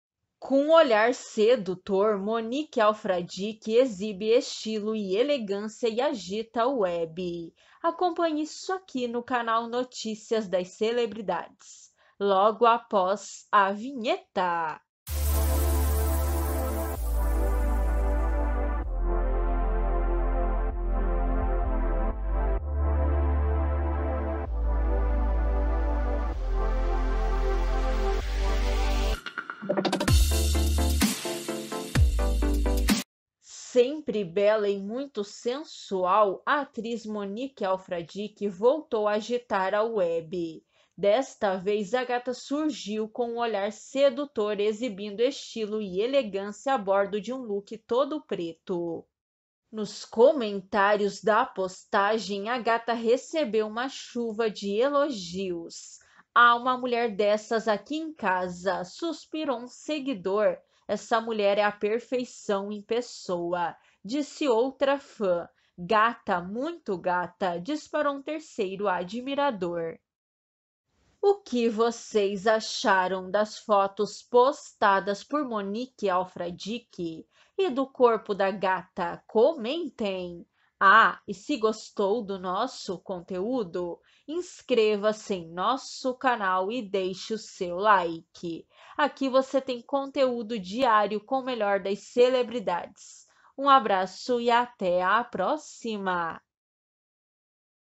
COM UM OLHAR SEDUTOR, MONIQUE ALFRADIQUE EXIBE ESTILO E ELEGÂNCIA E AGITA A WEB!
Com olhar sedutor, Monique Alfradique exibe estilo e elegância e agita a web!

Sempre bela e muito sensual, a atriz Monique Alfradique voltou a agitar a web. Desta vez, a gata surgiu com um olhar sedutor, exibindo estilo e elegância a bordo de um look todo preto.

Nos comentários da postagem, a gata recebeu uma chuva de elogios: “Ah uma mulher dessas aqui em casa!”, suspirou um seguidor; “Essa mulher é a perfeição em pessoa”, disse outra fã; “Gata, muito gata!”, disparou um terceiro admirador

Assistam o vídeo até o final e comentem o que vocês acharam das fotos postadas por Monique Alfradique! E do corpo da gata? Comentem!